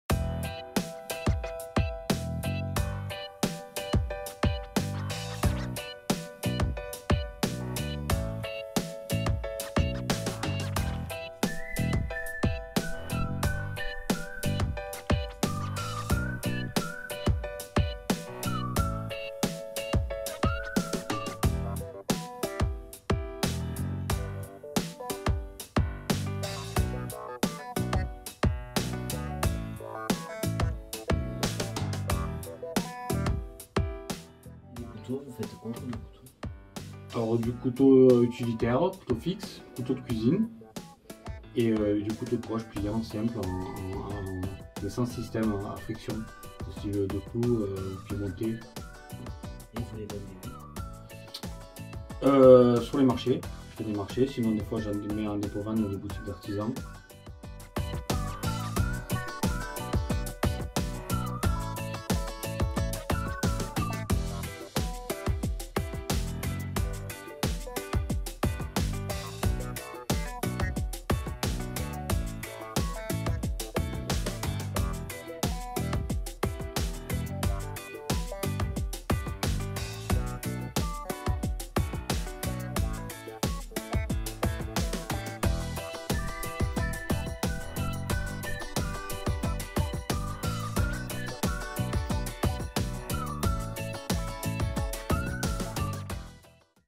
Les matinées du canton - Ayssènes
À Ayssènes, nous avons évoqué les travaux de rénovation du pont suspendu de Verdale, directement sur site avec la Maire, ses élus et Nadine Fraysse.
➡️Puis, nous avons visité le nouvel atelier de la Forge des Raspes pour découvrir des produits de grande qualité.
➡️J'ai été ravi de rencontrer l’association des chasseurs du territoire qui nous ont fait visiter l’aménagement de leur espace.